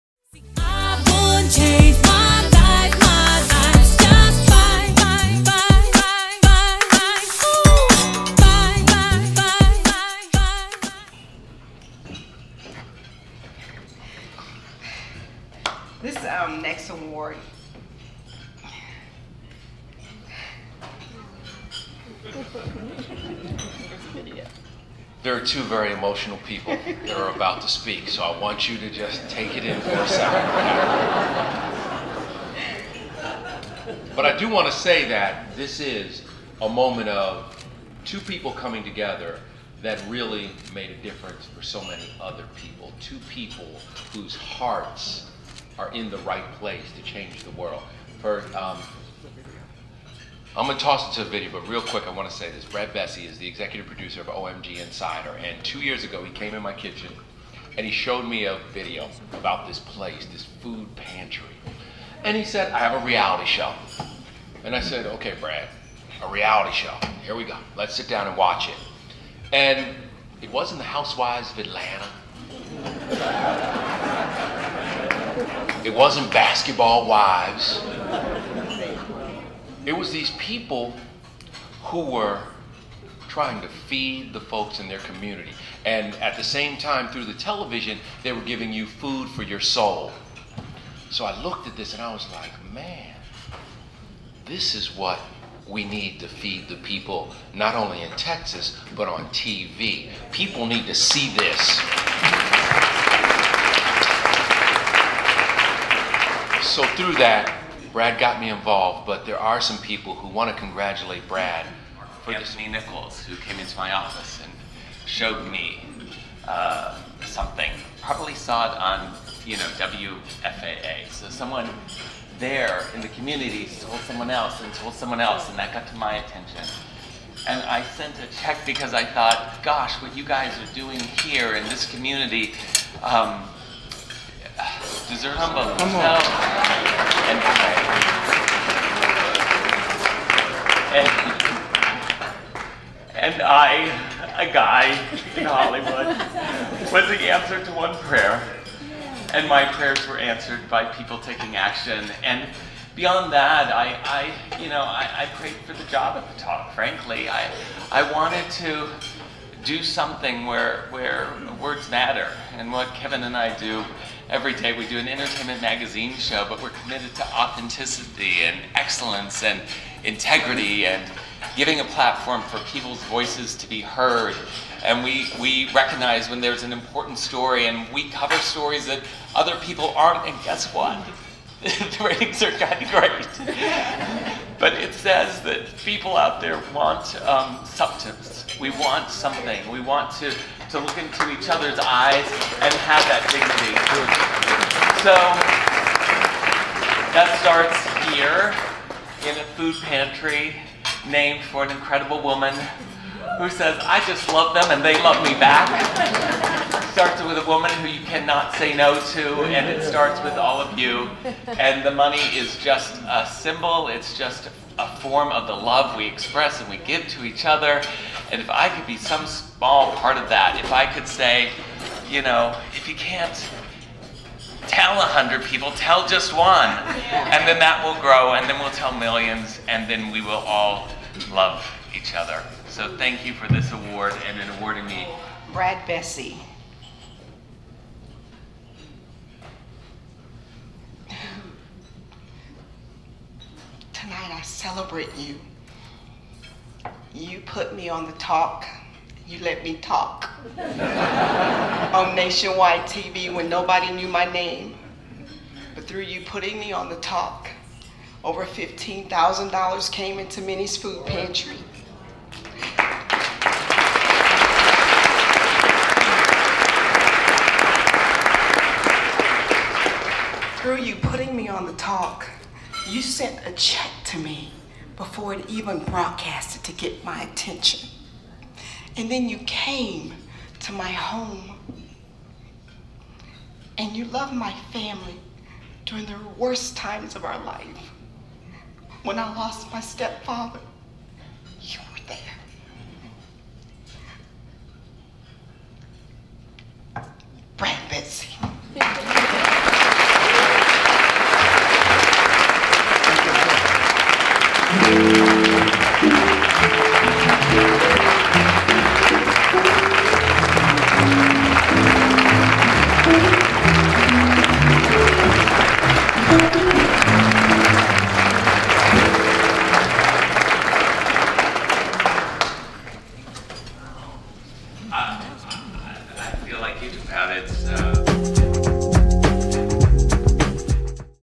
Here's what you missed, Minnies FP Gala 2013 w/Brad Bessey & Kevin Frazier
Minnie's Food Pantry gala 2013 celebration with Brad Bessey and Kevin Frazier of The Insider